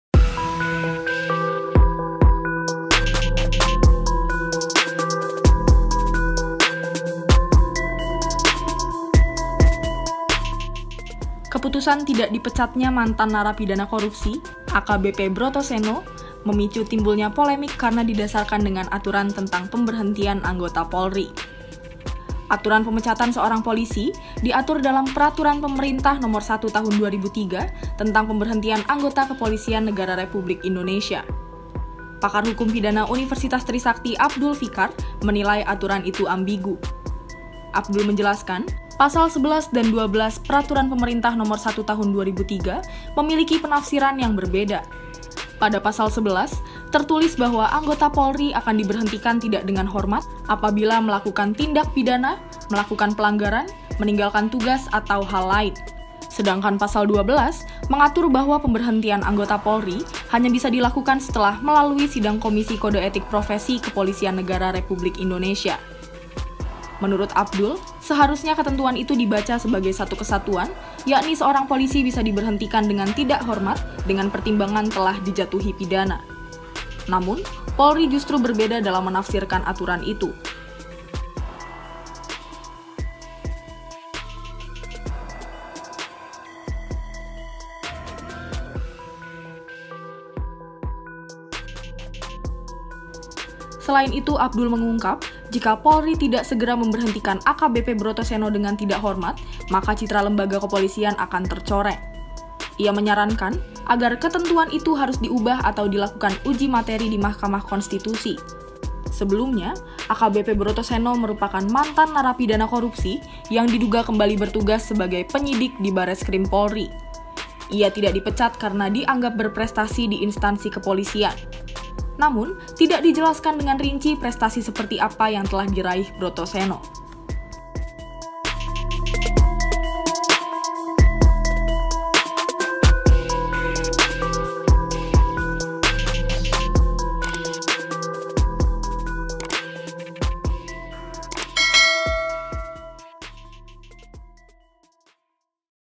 AKBP Brotoseno Belum Dipecat, Aturan Ini Dinilai Jadi Ambigu
Aturan pemecatan anggota Polri dianggap sebagai jadi pangkal masalah yang memicu polemik tentang AKBP Raden Brotoseno yang merupakan mantan narapidana korupsi tetapi kembali berdinas sebagai polisi.
Adapun aturan tersebut diatur dalam Peraturan Pemerintah (PP) Nomor 1 Tahun 2003 tentang Pemberhentian Anggota Kepolisian Negara Republik Indonesia.
Pakar hukum pidana Universitas Trisakti Abdul Fickar Hadjar mengatakan bahwa aturan tersebut ambigu.
Abdul menyarankan supaya ketentuan tersebut harus diubah atau dilakukan uji material (judicial review) di Mahkamah Konstitusi karena jika tidak dilakukan akan mencoreng citra lembaga kepolisian sendiri.
Penulis: Aryo Putranto Saptohutomo
Penulis Naskah: Chrisstella Efivania Rosaline
Narator: Chrisstella Efivania Rosaline
Video Editor: Timothy Afryano
Produser: Adisty Safitri